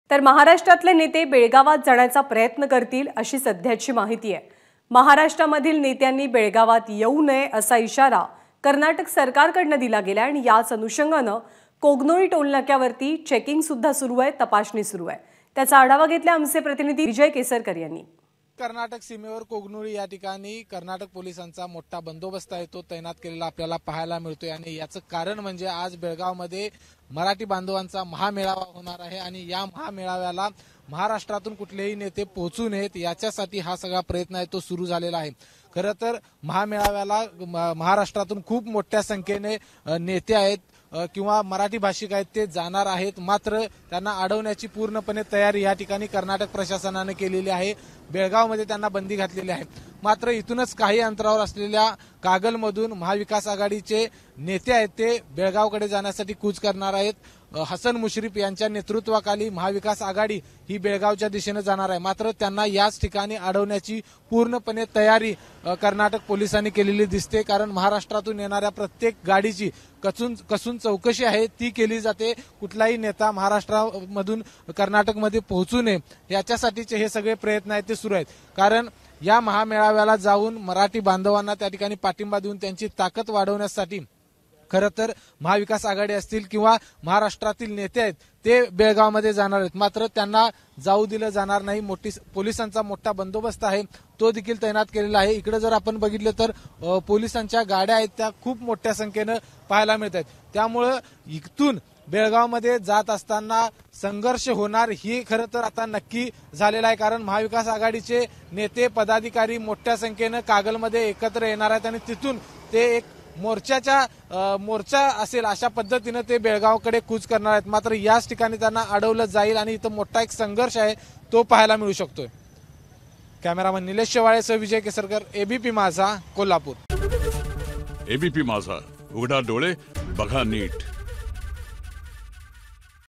Belgaum : बेळगावात मराठी बांधवांचा मेळावा, महाराष्ट्रातील नेत्यांना रोखण्यासाठी पोलिसांकडून नाकाबंदी
कर्नाटक विधानसभेचे हिवाळी अधिवेशन बेळगावात आजपासून सुरू होणार आहे. त्याला उत्तर म्हणून महाराष्ट्र एकीकरण समितीकडून व्हॅक्सीन मैदानावर मेळावा आयोजित करण्यात आलाय.   या मेळाव्यास महाराष्ट्रातील नेत्यांना आमंत्रित करण्यात आलय.कर्नाटक सरकारने मात्र महाराष्ट्रातील नेत्यांना प्रवेश देणार नसल्याच जाहीर केलय.